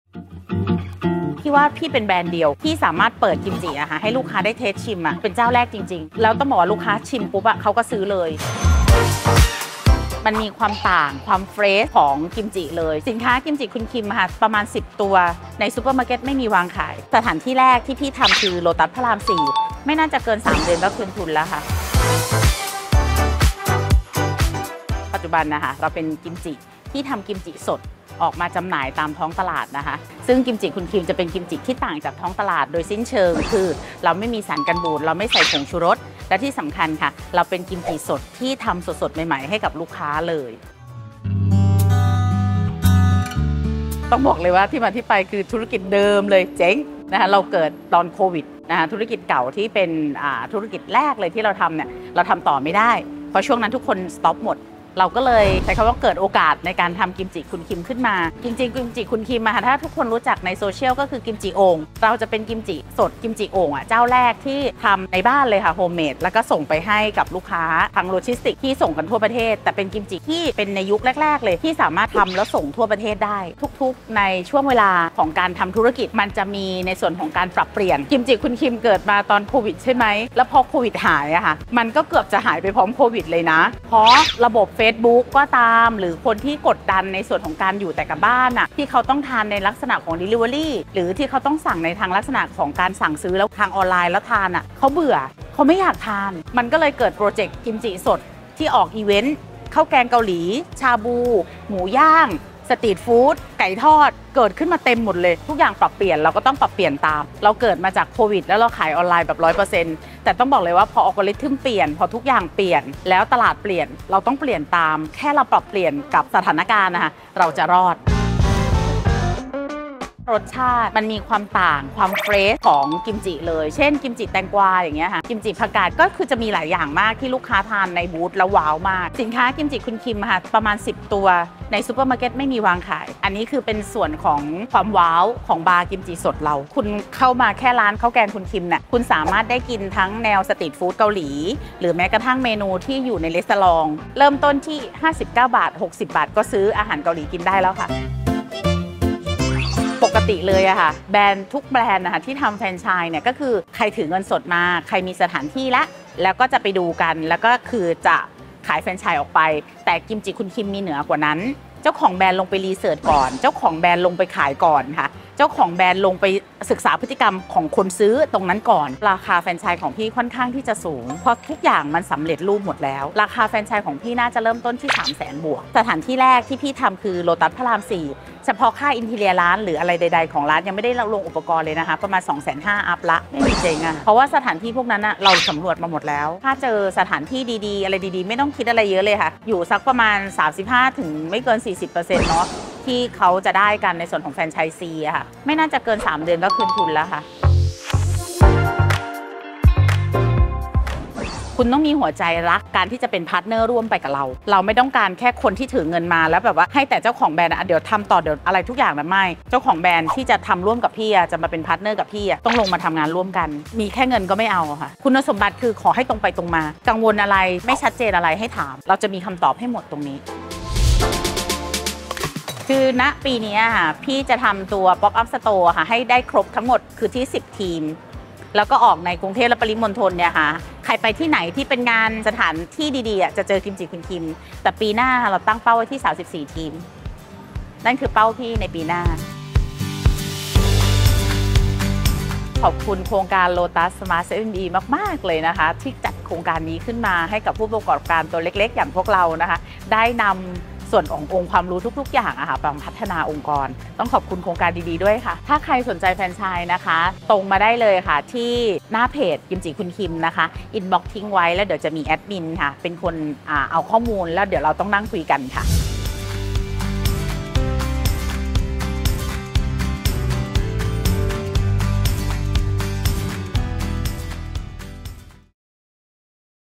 กิมจิผักกรอบโฮมเมด ซอสอร่อย ทำสดใหม่ทุกวัน มีให้เลือกกว่า 10 ชนิด
ร้านกิมจิคุณ Kim เป็นกิมจิผักกรอบแบบโฮมเมด ทำสดใหม่ทุกวัน มีให้เลือกกว่า 10 ชนิด ไม่ว่าจะเป็น กิมจิผักกาดขาว กิมจิแตงกวา หรือกิมจิหัวไชเท้า และอีกมากมาย นอกจากมีกิมจิแล้ว ที่นี่ยังมีข้าวแกงเกาหลี ไก่ทอด หมูย่างเกาหลี ให้ได้เลือกทานแบบจุใจ.